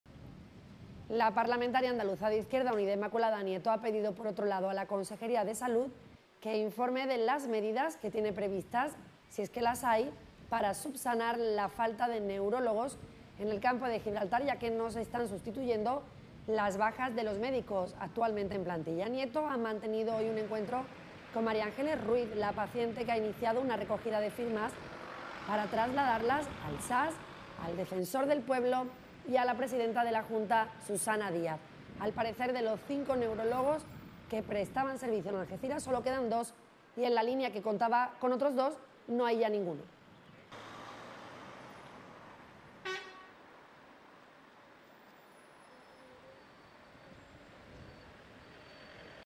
IU preguntará al consejero de Salud por la falta de neurólogos en el Campo de Gibraltar
La parlamentaria andaluza de Izquierda Unida y portavoz de IU en la Comisión de Salud del Parlamento, Inmaculada Nieto, presentará una pregunta dirigida a la Consejería de Salud para que informe de las medidas que tiene previstas, si es que las hay, para subsanar la falta de neurólogos en el Campo de Gibraltar ya que no se están sustituyendo las bajas de los médicos actualmente en plantilla. Nieto ha mantenido hoy un encuentro con María Ángeles Ruiz, la paciente que ha iniciado una recogida de firmas para ser oída por el SAS, el Defensor del Pueblo y Susana Díaz. La falta de neurólogos en este centro hospitalario complica considerablemente la atención a los pacientes. Al parecer de los cinco neurólogos que prestaban servicio en Algeciras y dos en La Línea, se ha pasado a cero en La línea y únicamente a dos en nuestra ciudad.